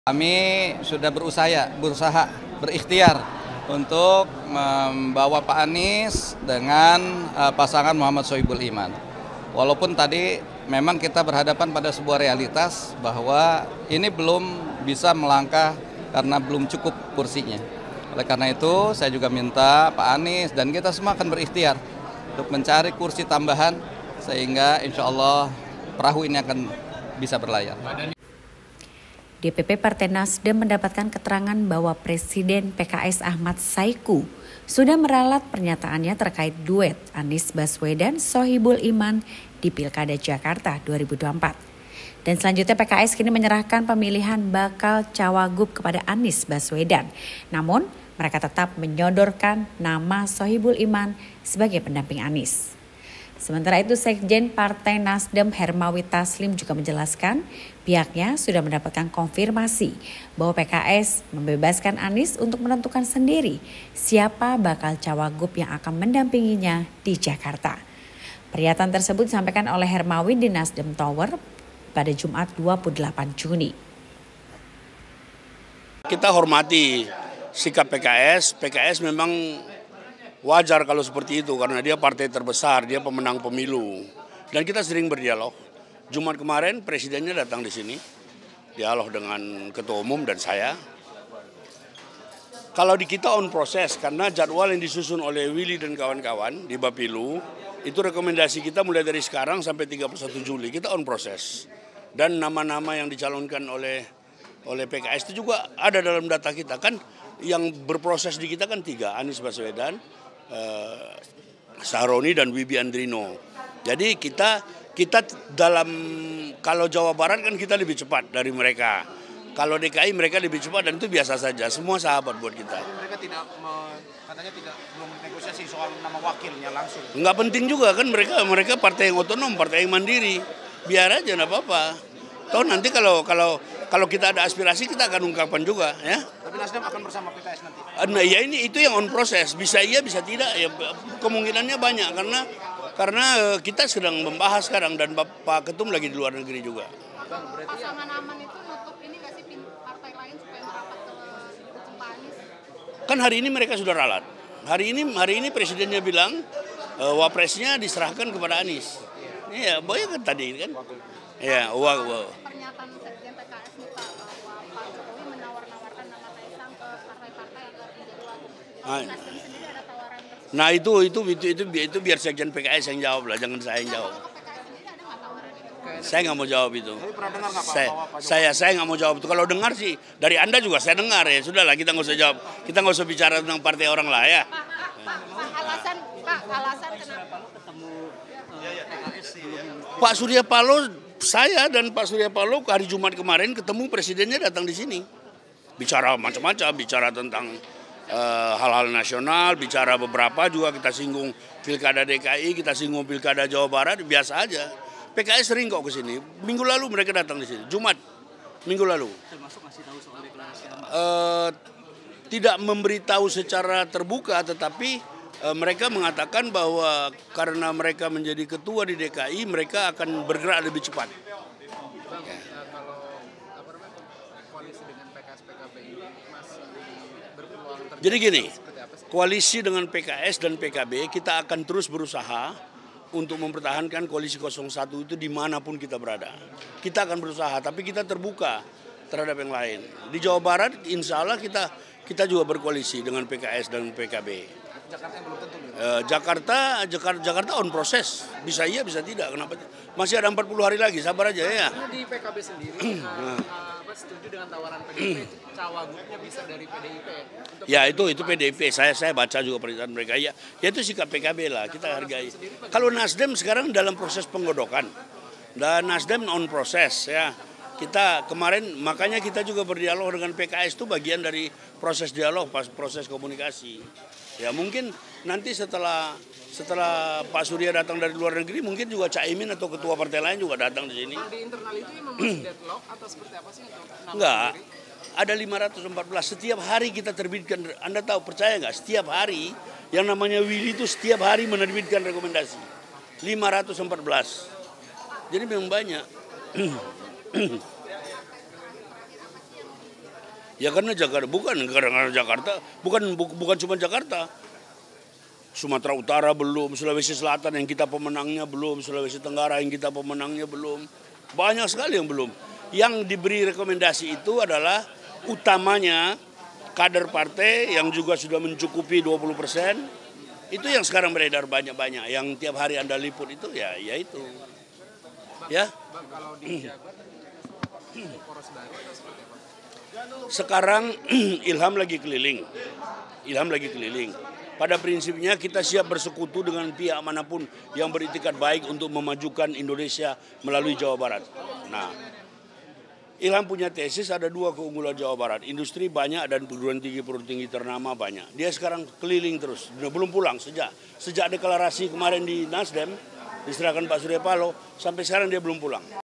Nasdem Bocorkan PKS “Batalkan” Sohibul Iman Jadi Cawagub Anies di Pilkada Jakarta,
WARTAKOTALIVE.COM, Jakarta -- DPP Partai NasDem mendapat keterangan bahwa Presiden PKS Ahmad Syaikhu telah meralat pernyataannya terkait duet Anies Baswedan-Sohibul Iman di Pilkada Jakarta 2024.
Selanjutnya PKS kini menyerahkan pemilihan bakal Cawagub kepada Anies Baswedan, namun mereka tetap menyodorkan Sohibul Iman sebagai pendamping Anies.

Video Editor: Luttex